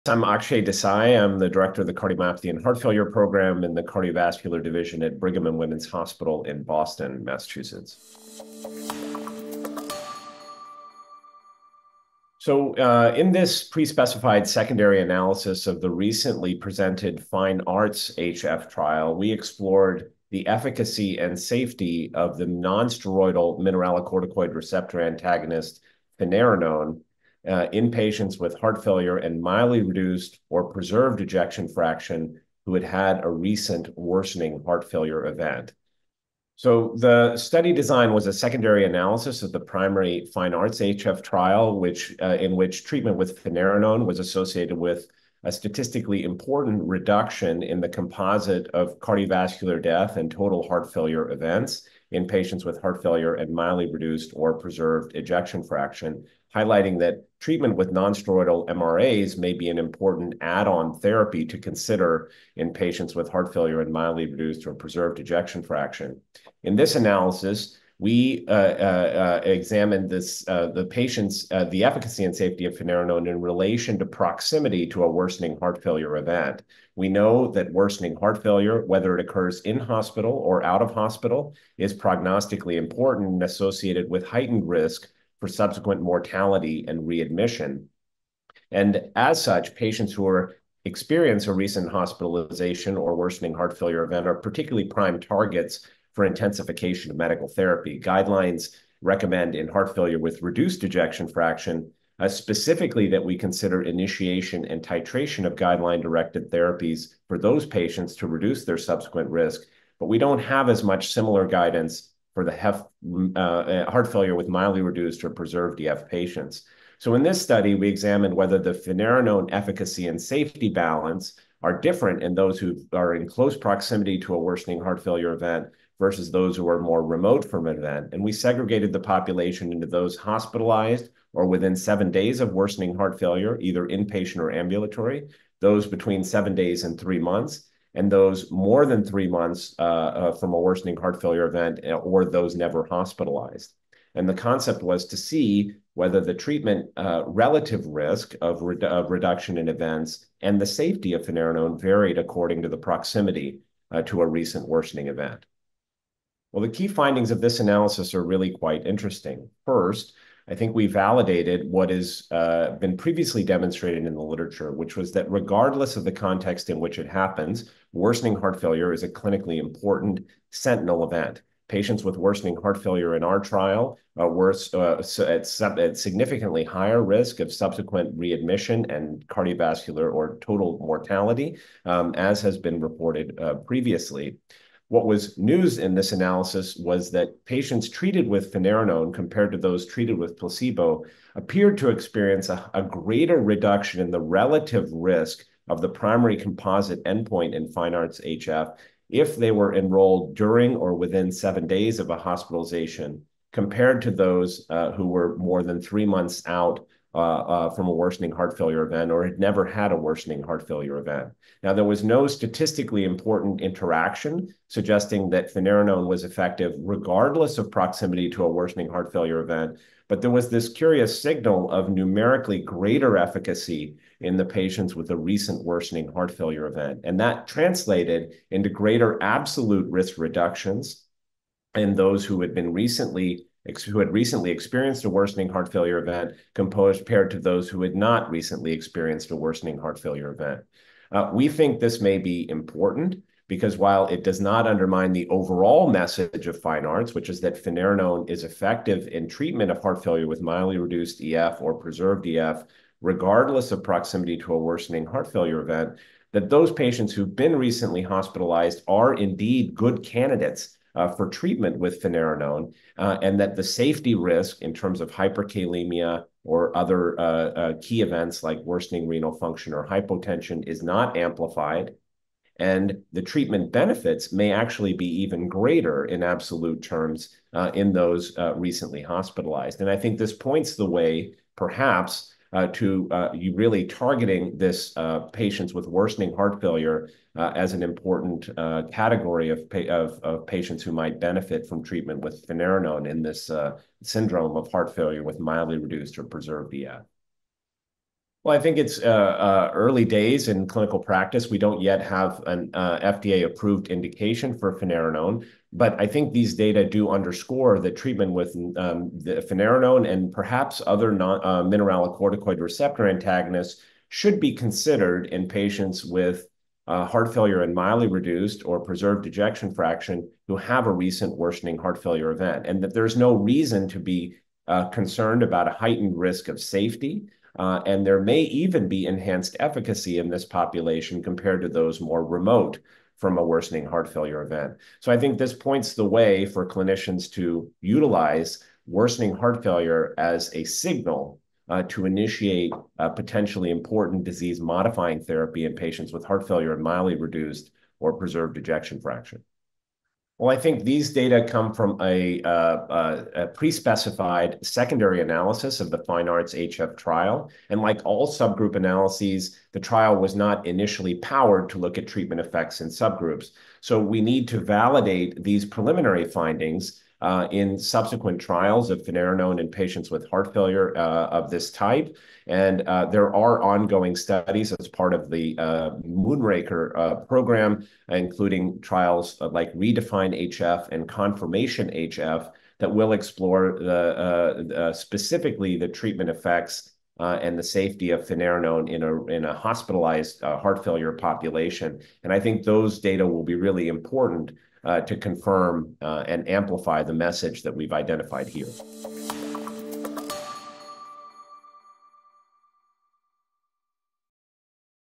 HFSA 24: Finerenone Across the Ejection Fraction Spectrum: FINEARTS-HF
At HFSA24, Dr Akshay Desai shares key findings from the FINEARTS-HF study, exploring finerenone's role across the ejection fraction spectrum in HeartFailure.

Dr Akshay Desai breaks down:
📊 Study rationale, design and patient population
🥼 Key findings on finerenone in heart failure
🩺 The potential impact on clinical practice